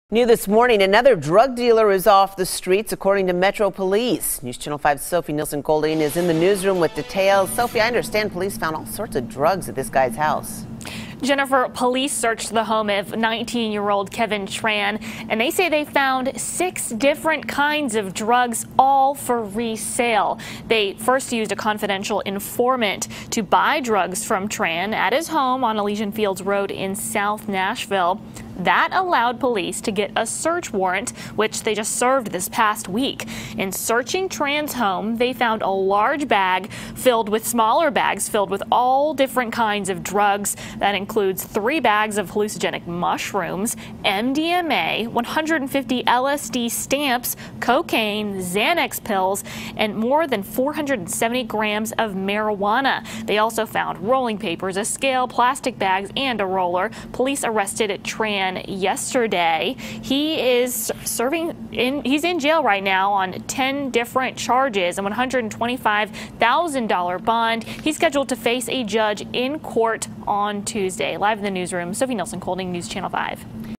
Alleged Drug Dealer Arrested On 10 Charges
Another drug dealer has been taken off the streets, according to those with the Metro Nashville Police Department.